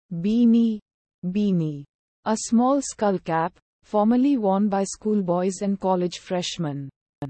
'Beany' Meaning and Pronunciation
Word: Beany
Meaning: a small skullcap; formerly worn by schoolboys and college freshmen